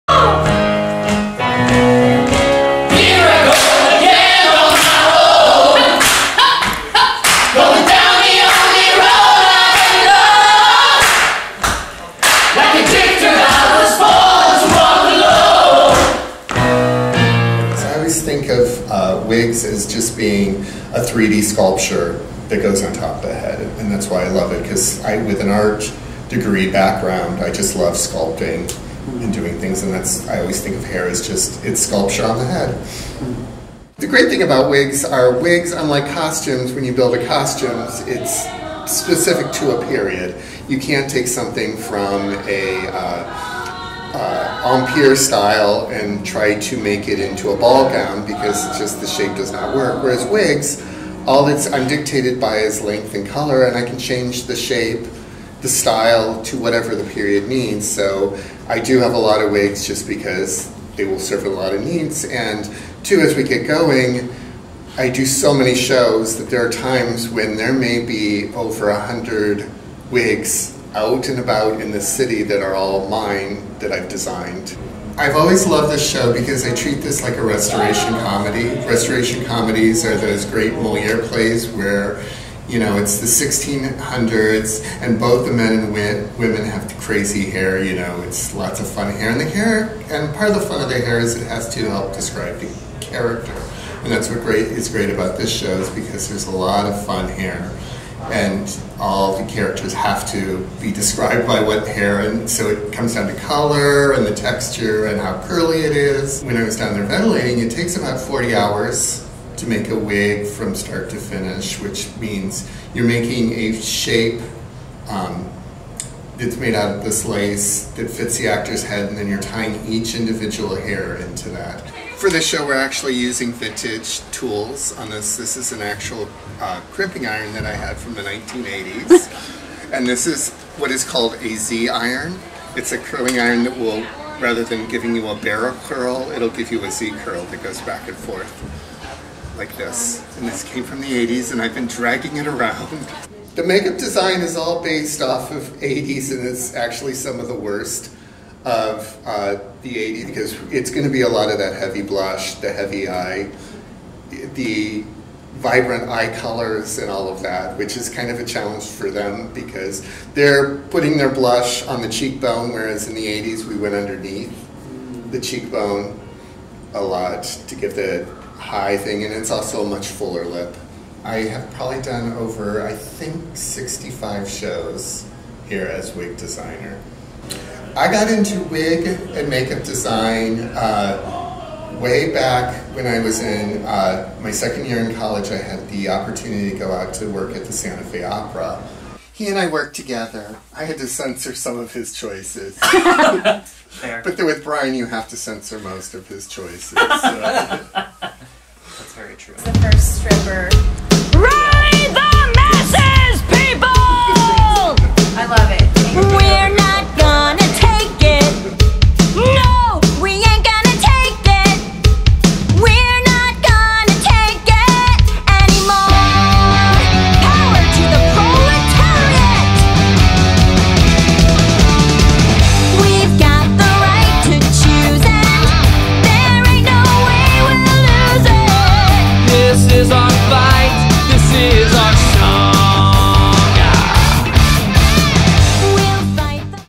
Cygnet in process: wig and makeup design
Behind the scenes with Peter Herman on Rock of Ages